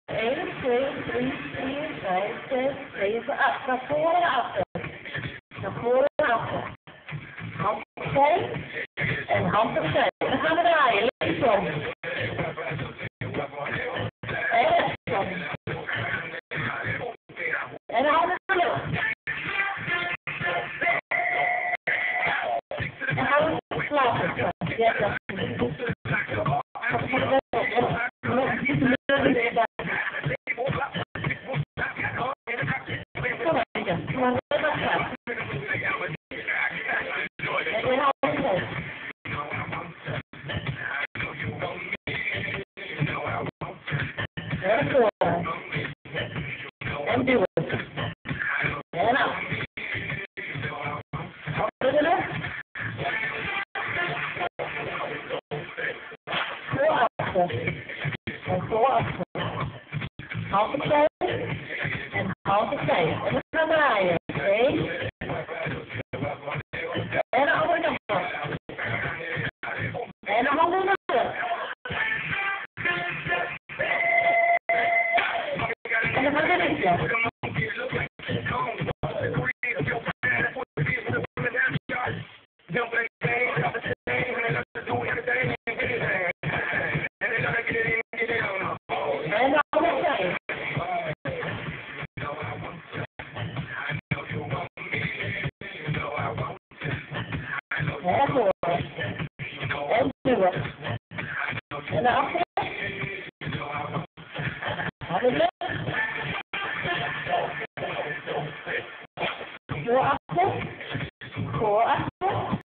Oefenen voor Hofdance 2010
Joy on Wheels aan het oefenen voor Hofdance 2010 in Goor.